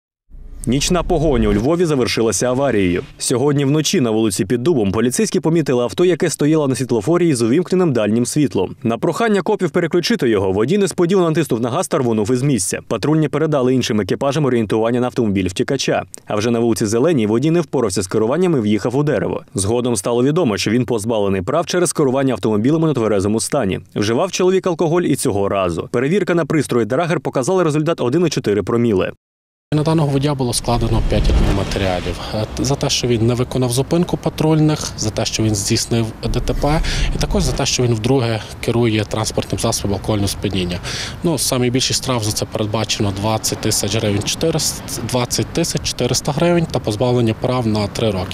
Нічна погоня у Львові завершилася аварією.
Сьогодні вночі на вулиці Під Дубом поліцейські помітили авто, яке стояло на світлофорі із увімкненим дальнім світлом. На прохання копів переключити його, водій несподівано натиснув на газ та рвонув із місця. Патрульні передали іншим екіпажам орієнтування на автомобіль утікача. Його переслідували аж шість екіпажів поліції. А вже на вулиці Зеленій водій не впорався з керуванням і в’їхав у дерево